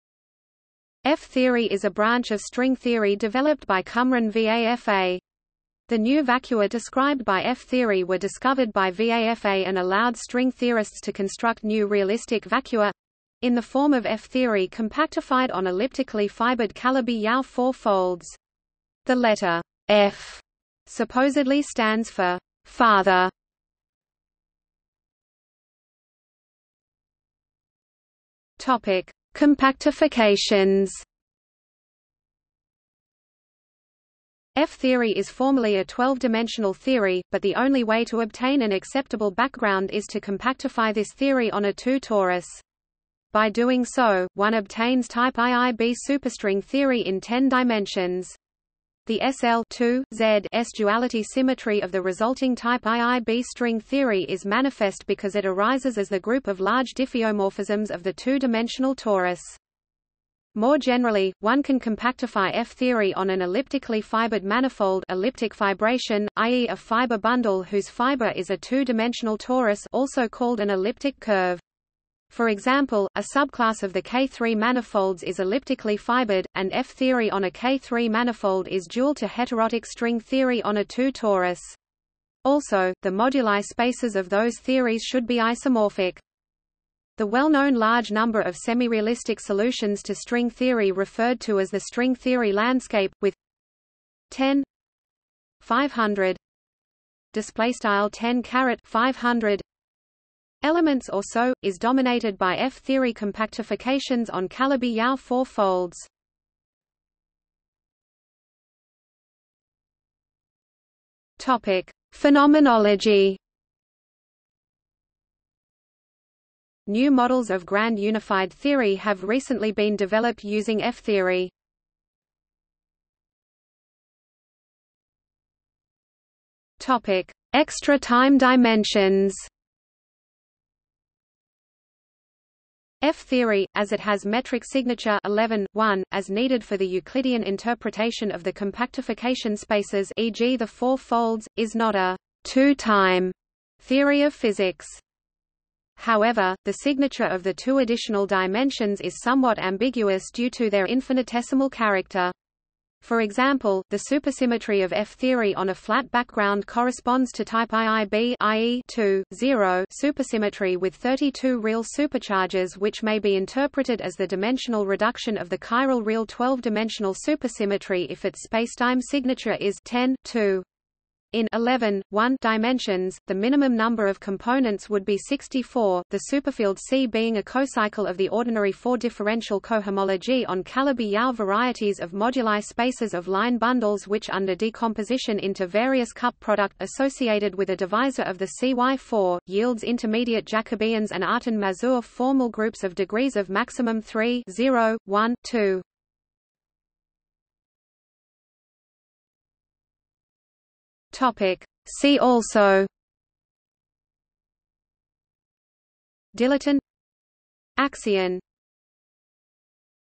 F-theory | Wikipedia audio article
00:00:25 1 Compactifications
00:01:47 2 Phenomenology
00:02:03 3 Extra time dimensions
00:03:17 4 See also

- Socrates

SUMMARY
F-theory is a branch of string theory developed by Cumrun Vafa. The new vacua described by F-theory were discovered by Vafa and allowed string theorists to construct new realistic vacua — in the form of F-theory compactified on elliptically fibered Calabi–Yau four-folds. The letter "F" supposedly stands for "Father".